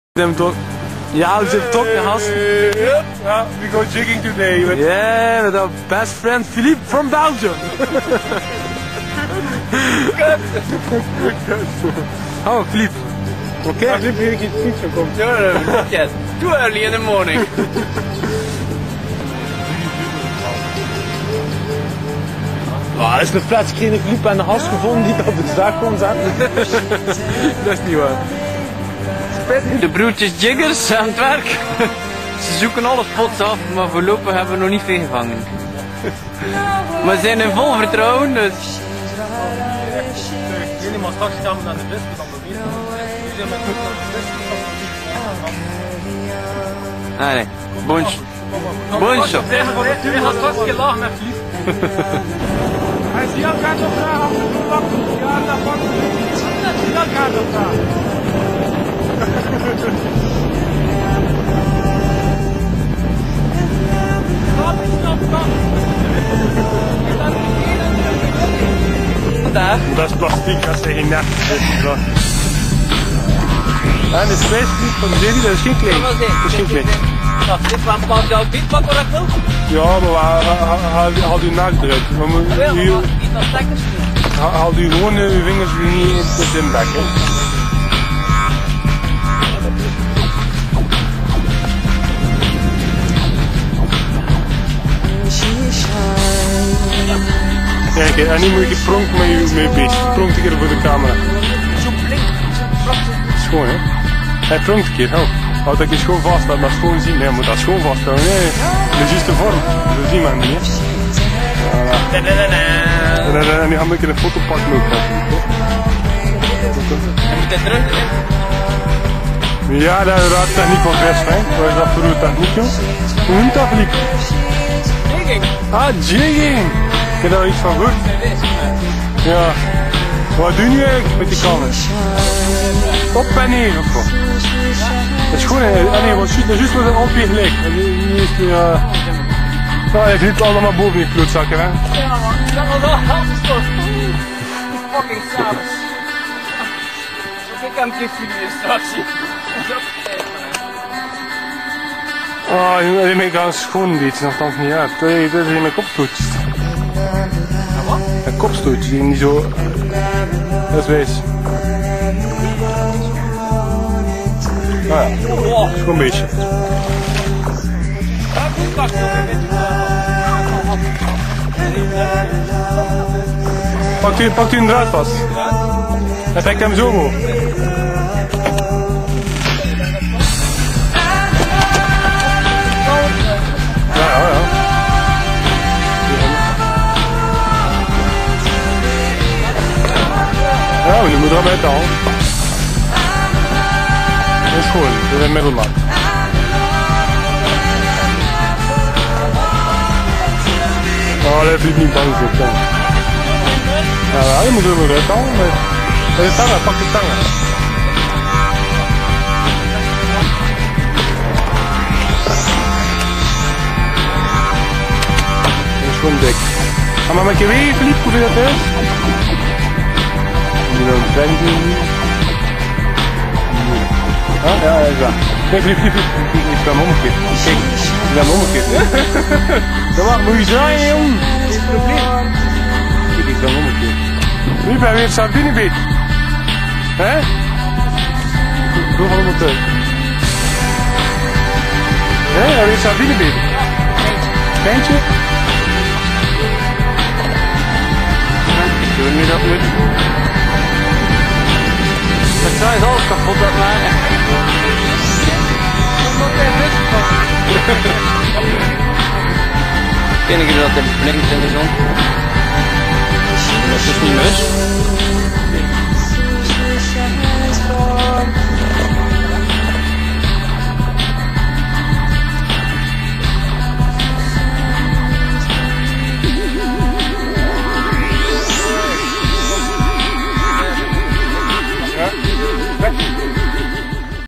JIGGING SABRES -250M ジギング太刀魚 SEADUDES
jigging trip with our best friend Philipe.